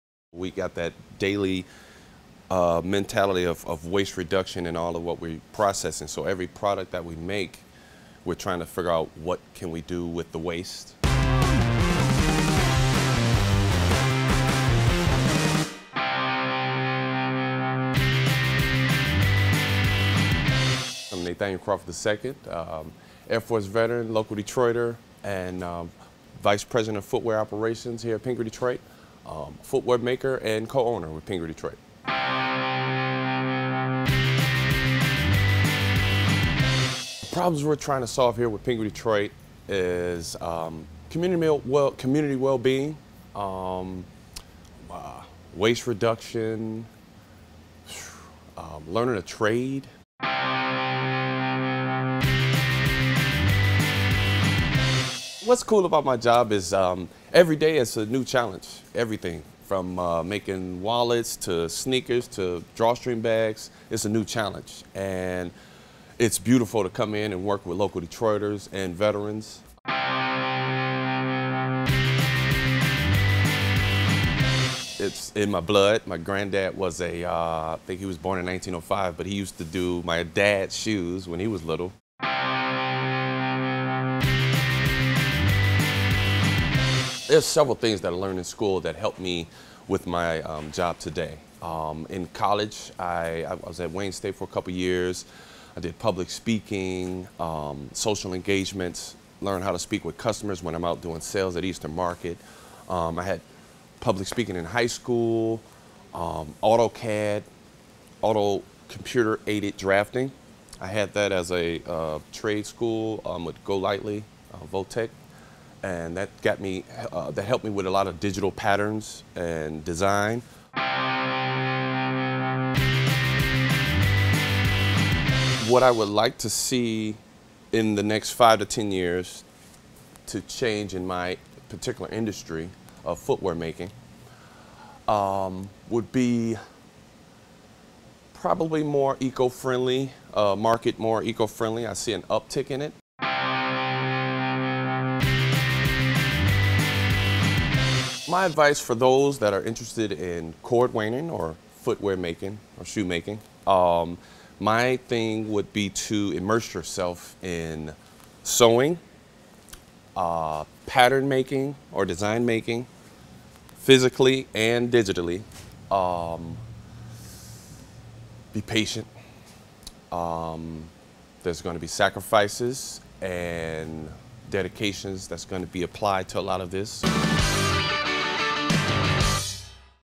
Vice President of Footwear Operations Nathaniel Croft | Career Chat
Vice President of Footwear Operations at Pingree Detroit, Nathaniel Croft II creates handmade footwear using recycled leather from Detroit’s auto industry.